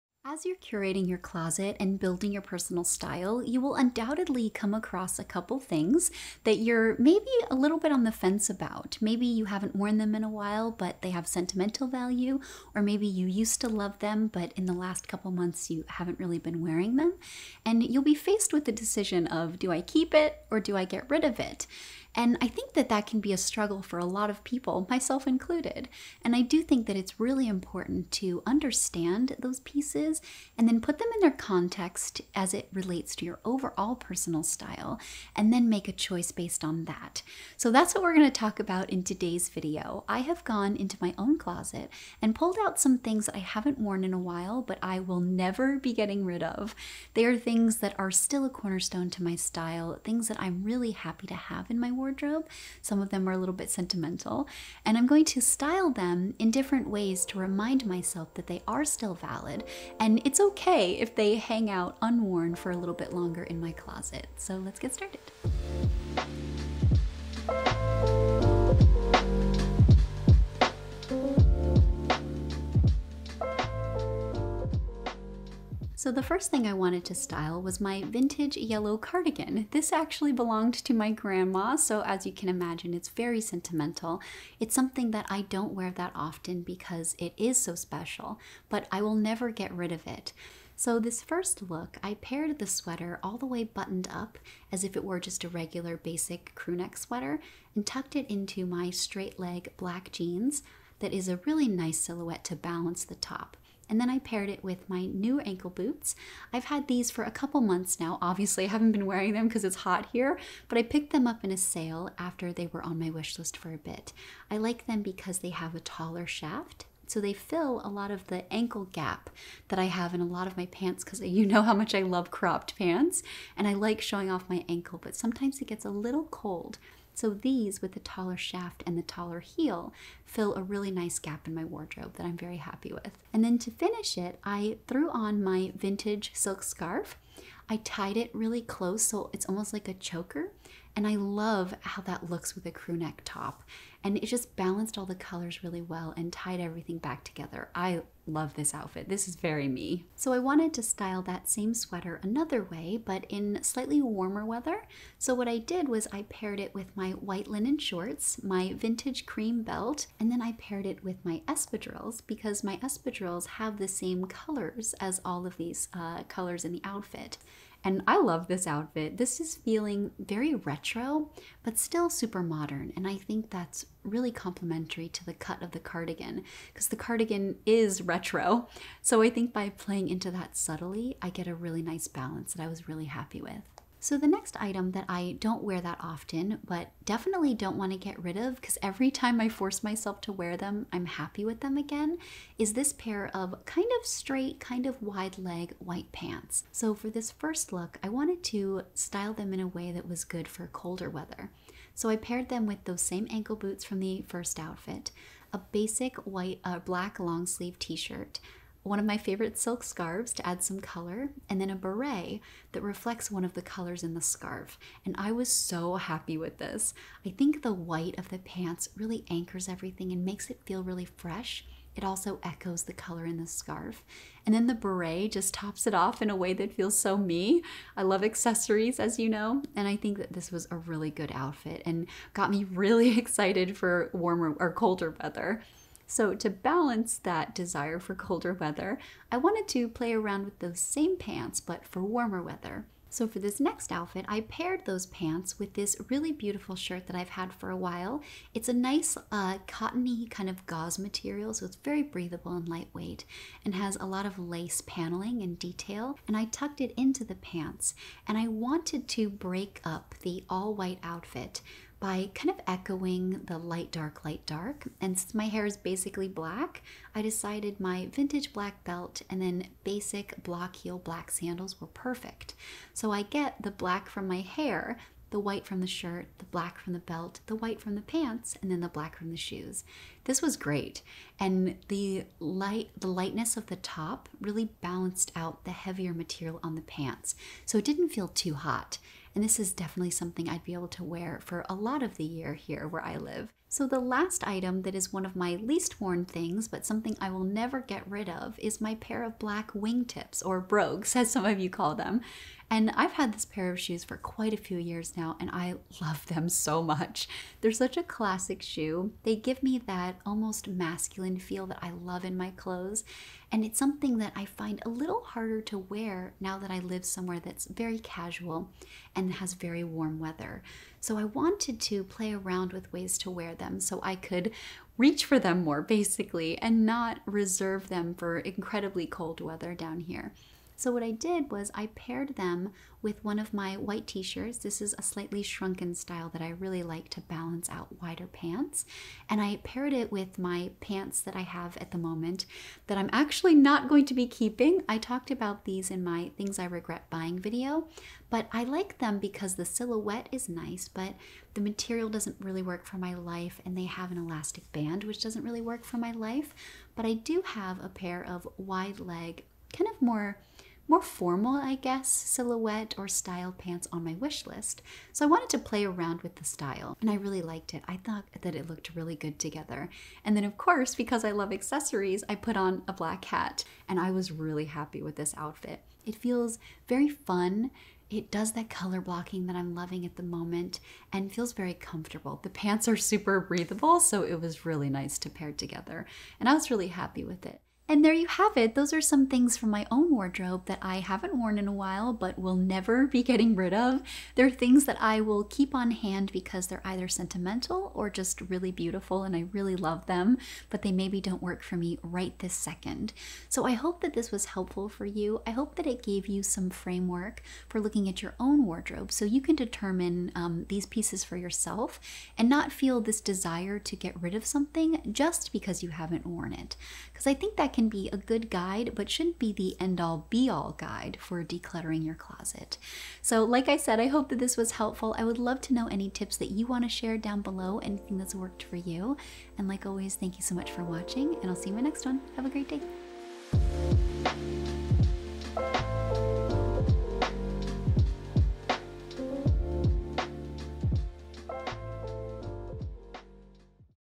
Styling Clothes I Never Wear But Won't Get Rid Of
Today I'm styling my least worn clothes. These are clothes I never (or hardly) wear but won't get rid of because they're sentimental or a key piece of my personal style that I know I'll circle back to. I hope this helps you identify anything in your own closet that may be waiting for it's day to shine but isn't worth decluttering.

ITEMS FEATURED & OUTFITS

Vintage Yellow Sweater From My Grandma
Vintage Silk Scarf - Etsy has so many
Agolde Black Jeans (I size down in Agolde and have these in a 24)
Aquatalia Black Ankle Boots - bought in the sale and no longer available but similar and here on sale with a lower heel I take my usual size 37
Vintage Cream Belt - vintage belts are the best!
Very old White Linen Shorts
Castaner Espadrilles (I recommend sizing up if you're between sizes & I take a 37)

Everlane White Wide Leg Pants (Some troubling information about Everlane has been brought to my attention and while it appears they're working to make real change and lasting progress, I encourage you to look into it more before deciding to buy from them. I will of course continue to wear my Everlane pieces because I'm a firm believer in using what you have but I will refrain from linking directly until improvements are made)
Very old, cotton, long sleeve black t-shirt but you can find these anywhere. I like cotton because it's breathable so I can layer under jackets and sweaters.
Sabina Savage Silk Scarf *so generously gifted but you know how much I love this brand and have purchased my own in the past
Aquatalia Black Ankle Boots - bought in the sale and no longer available but similar and here on sale with a lower heel
Sezane White Shirt - from a few months ago, no longer available but they have so many beautiful tops and wonderful quality and materials (I always size down because they run large - I'm wearing XS here and as you can see it's still big)
Black Vintage Belt
Everlane Black Block Heel Sandals - See my note above about Everlane

Black Brooks Brother's Wingtips, from about 7 years ago and I've sadly seen a decline in the quality of their shoes since then - Similar options here and here with a thicker sole that I love for a larger investment with great quality.
Cropped and shrunken white t-shirt - you can find these everywhere
Vintage sunglasses
Thrifted hat so I can test the style before investing in it